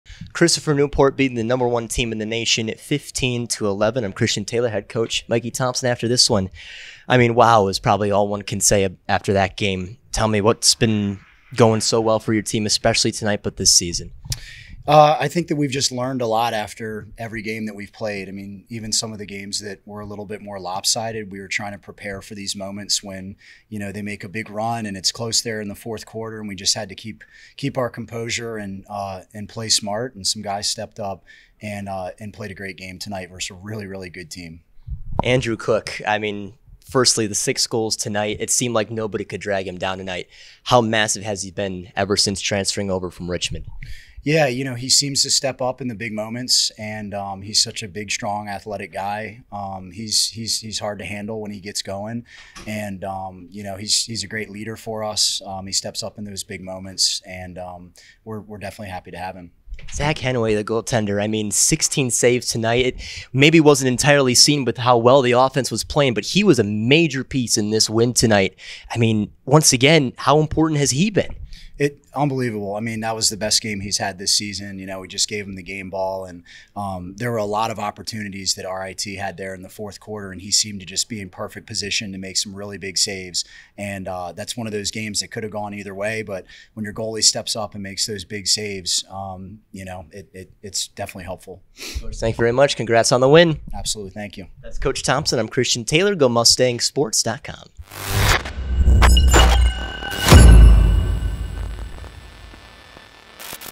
2023 Mustang Classic Christopher Newport vs RIT Postgame with Mikey Thompson
Christopher Newport head coach Mikey Thompson talks about the team's game against RIT in the 2023 Mustang Classic.